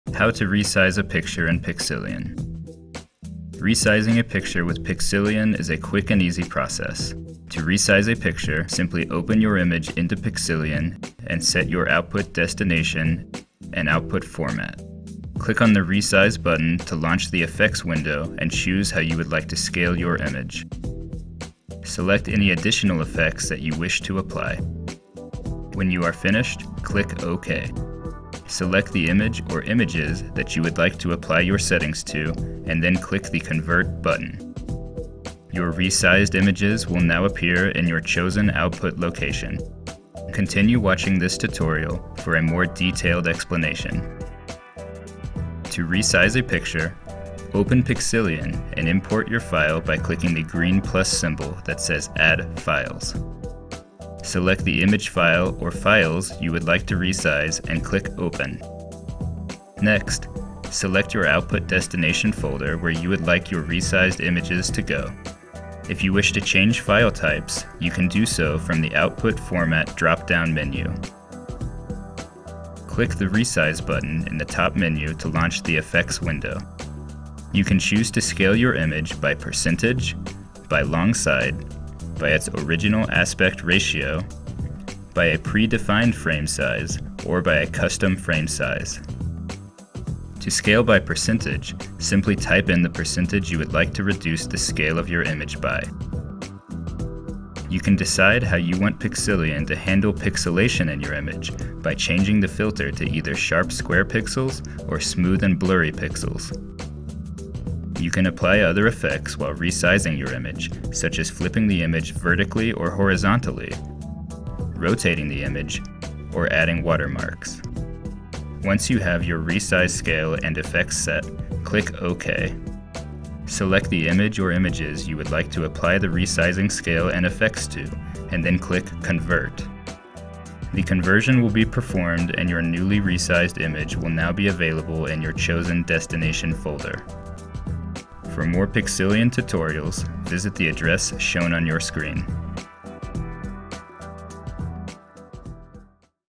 How to Resize an Image | Pixillion Image Converter Tutorial
This video will show you how to resize a picture in Pixillion.

Resizing a picture with Pixillion is a quick and easy process.

To resize a picture, simply open your image into Pixillion and set your output destination and format. Click on the resize button to launch the effects window and choose how you would like to scale your image. Select any additional effects that you wish to apply. When you are finished click OK. Select the image or images that you would like to apply your settings to and then click the Convert button. Your resized images will now appear in your chosen output location.

For more Pixillion tutorials visit the address shown on your screen.